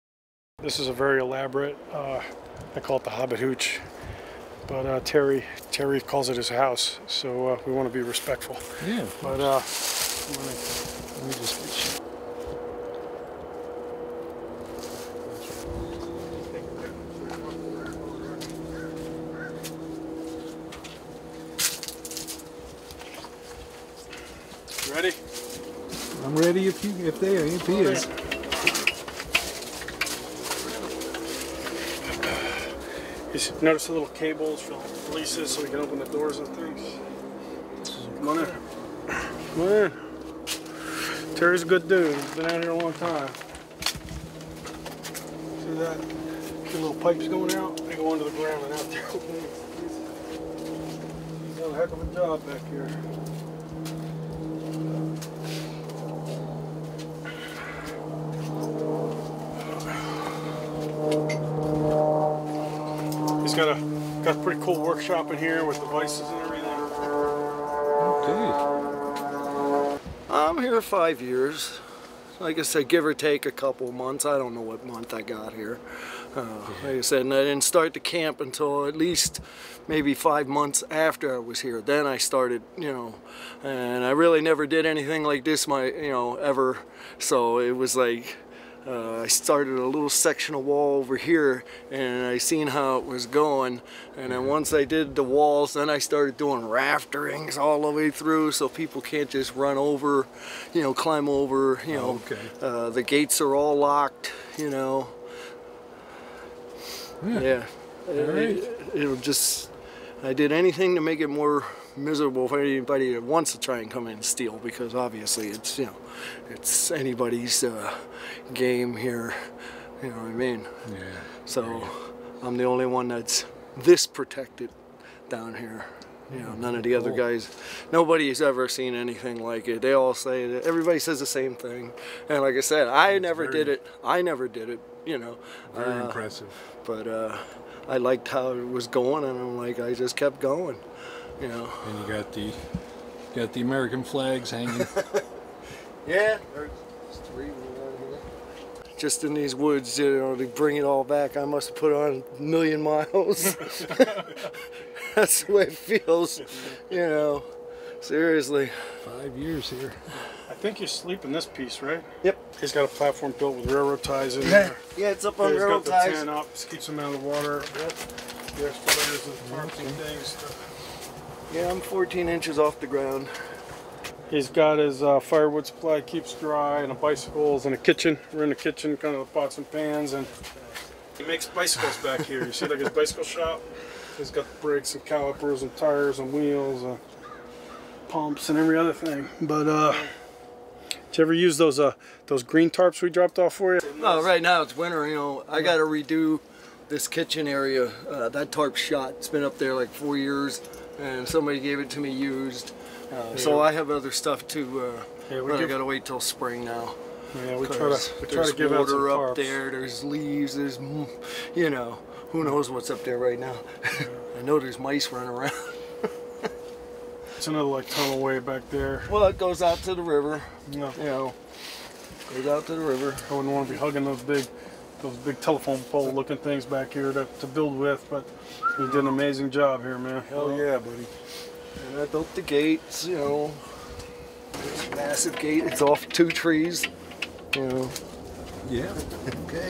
Homeless Terry and the "Hobbit Hooch"
Mark Baylis of the Valor Clinic Foundation give us a quick tour of the "Hobbit Hooch" as he calls it. One of the homeless that he and the foundation help, Terry, put this shelter together to get a little protection.

Another resident of a homeless camp which is one of many Mark visits in his mission to provide assistance accessing benefits, and shelter to Veterans with limited or no access to care and shelter to Veterans who due to health or means are unable to find or keep shelter of their own and Post Traumatic Stress resources to Veterans through their Veterans Unstoppable program.